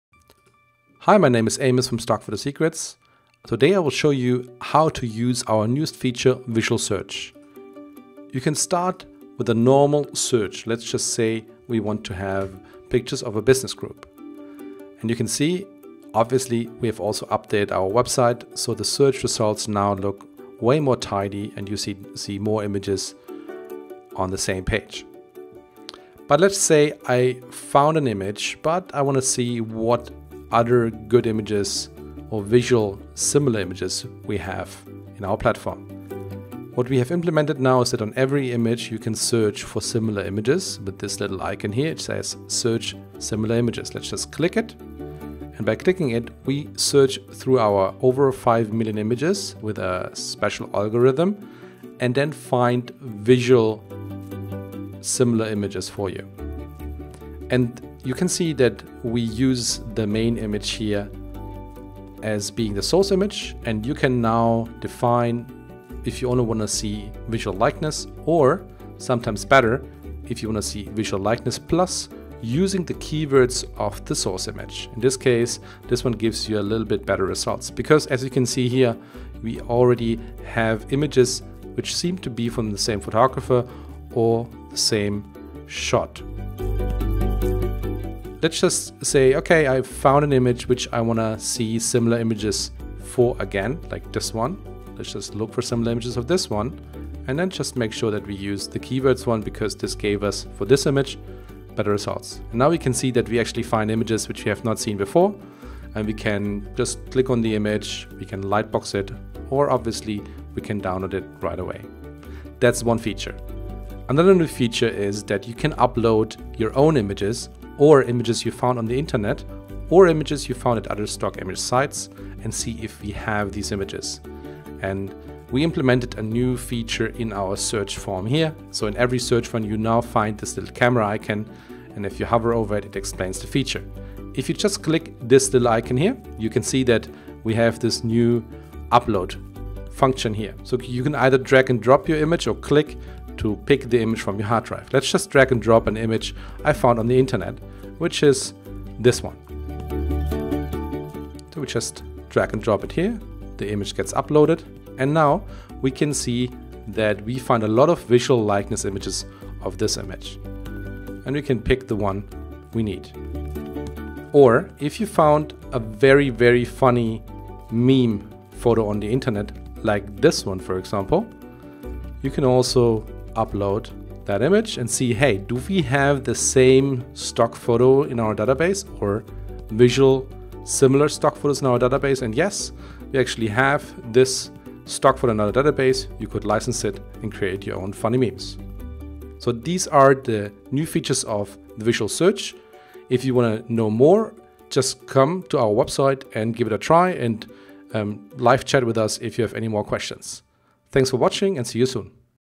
How to find Images with Visual Search on Stock Photo Secrets Shop
We explain in detail how to use our new visual search engine to find the perfect stock photo for your project.